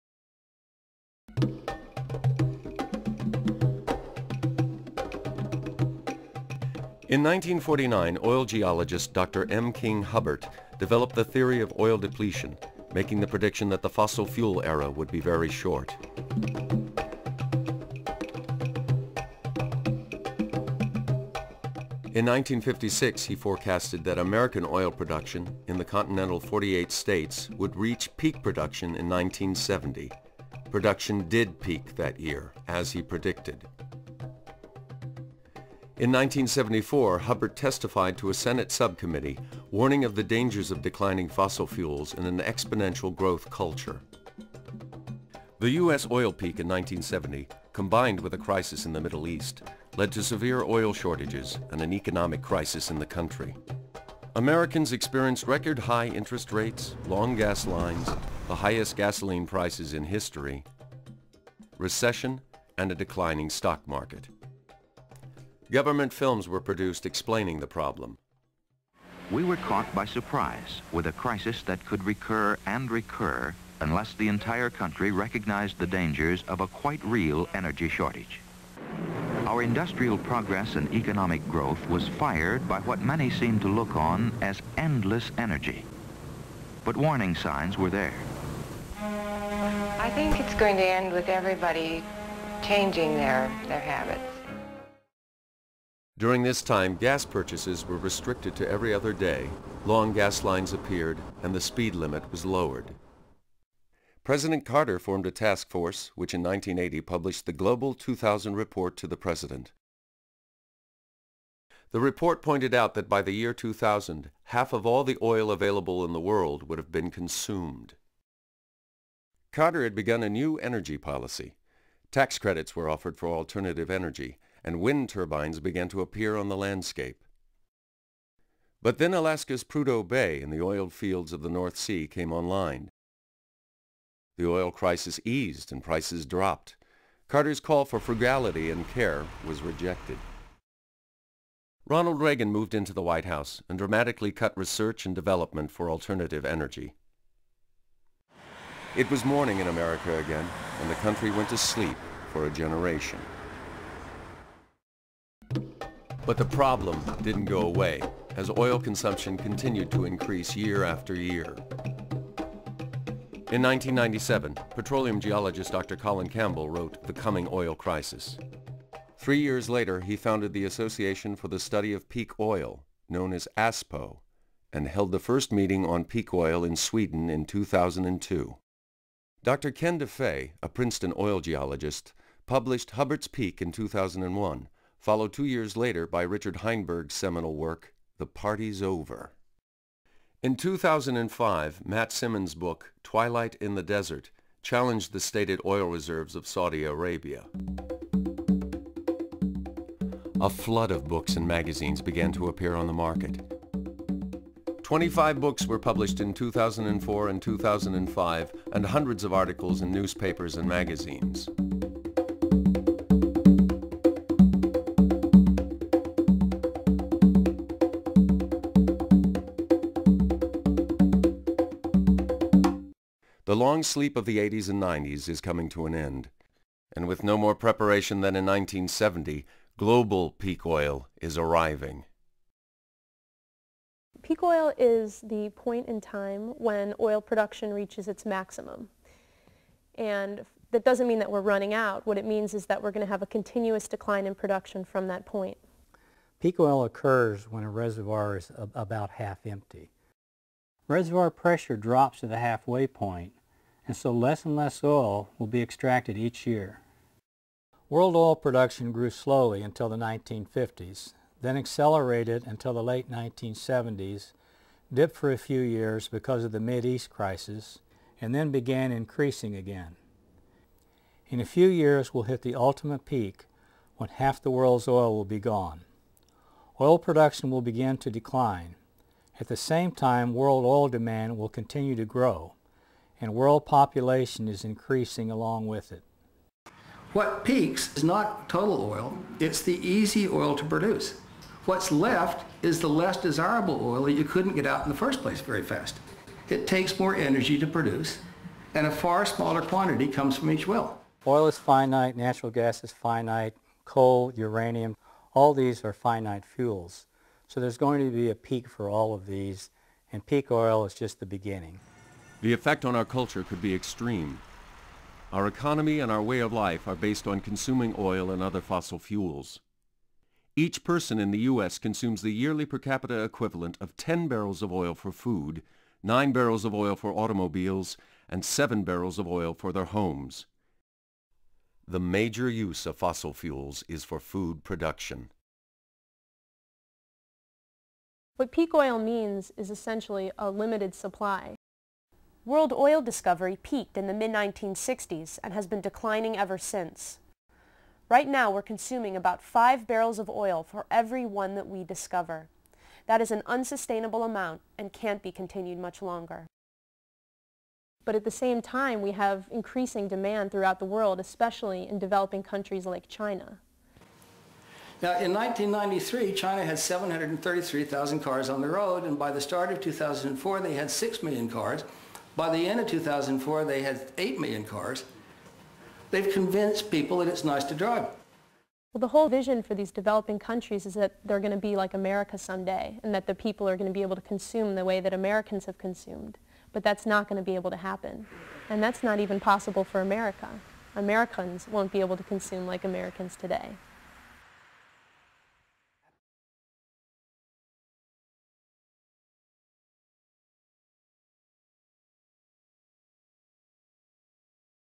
When the oil runs out
A video for the UMass Sustainable Living class on peak oil.